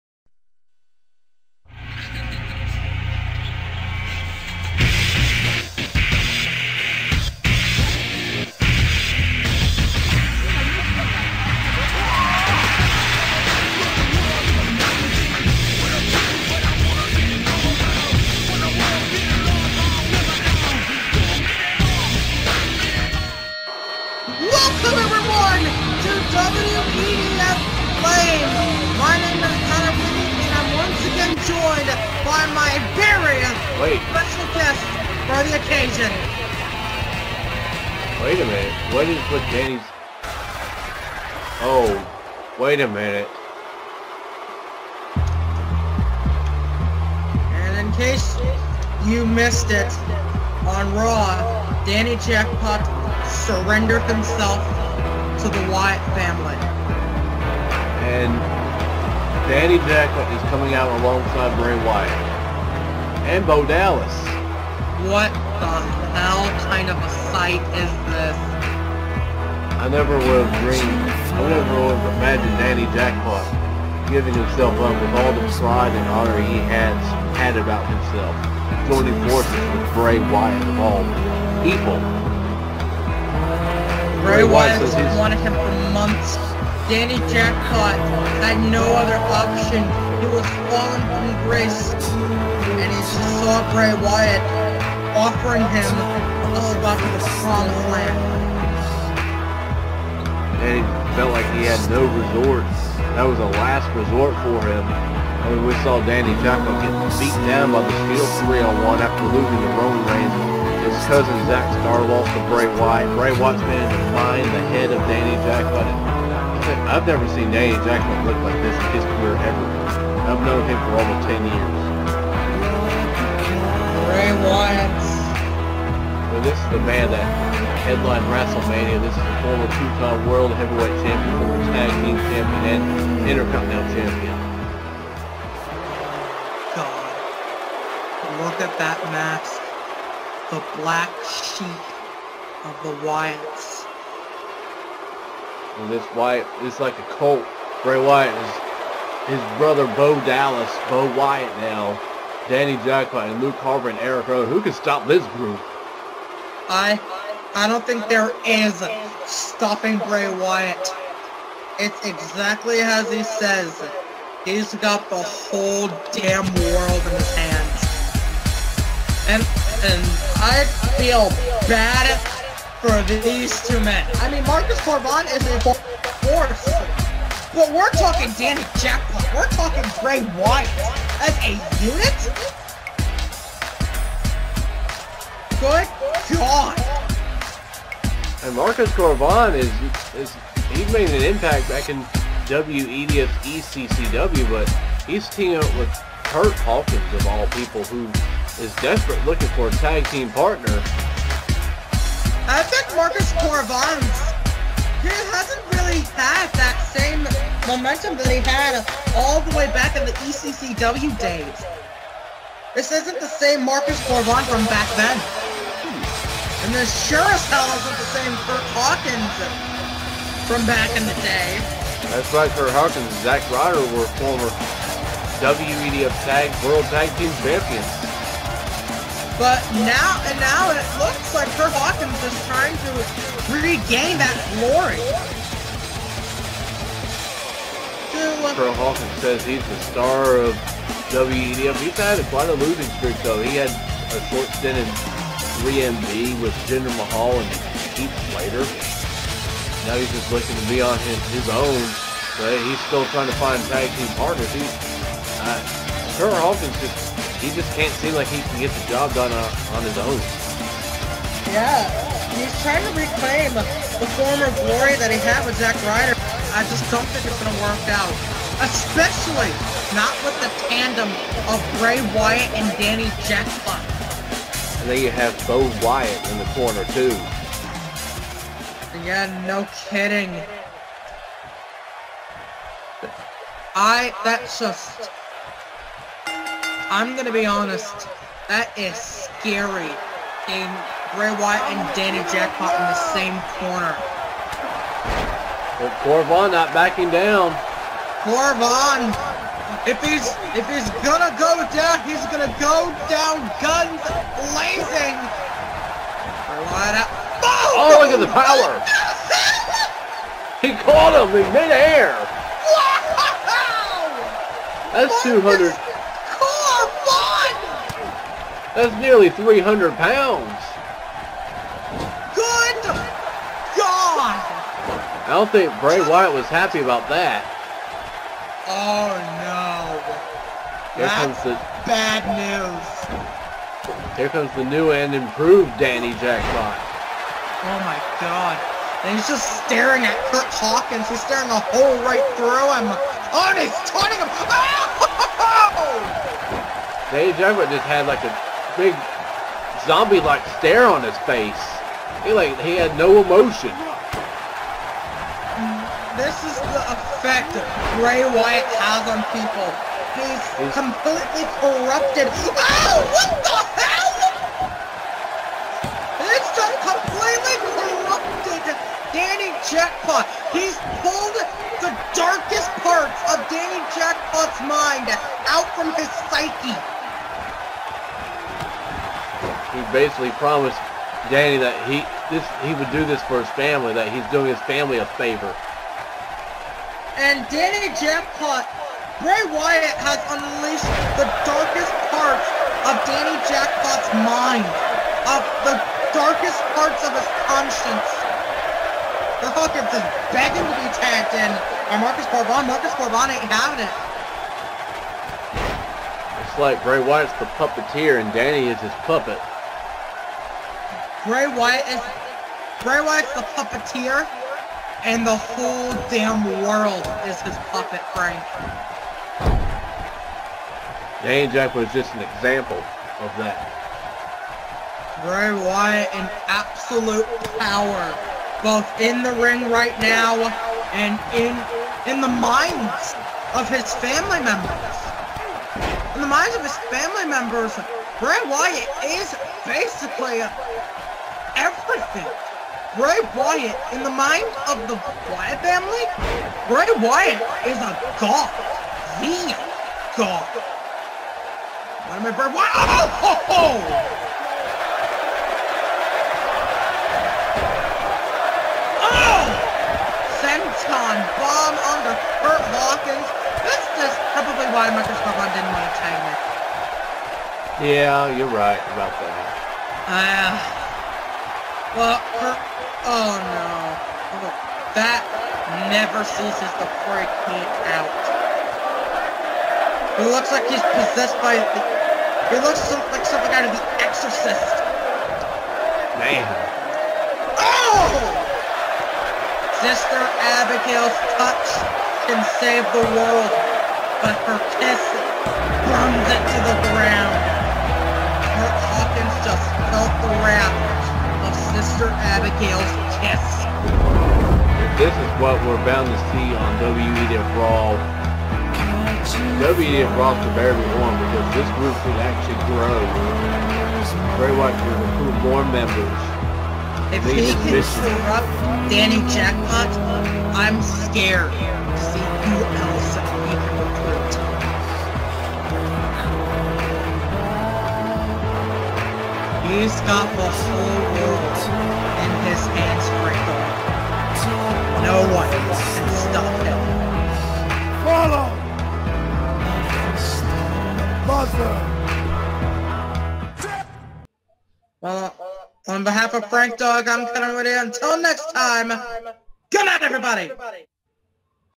WEDF Flame: 5-20-2018 - Bray Wyatt and Danny Jackpot vs. Marcus Cor Von and Hawkins
Bray Wyatt and his new disciple, the CAW's Icon Danny Jackpot are in tag team action alongside Bray's brother Bo Dallas as they take on the strange tag team of Marcus Cor Von and Curt Hawkins on WEDF Flame as Hawkins quest for a tag team partner begins.  How will Danny and Bray work together?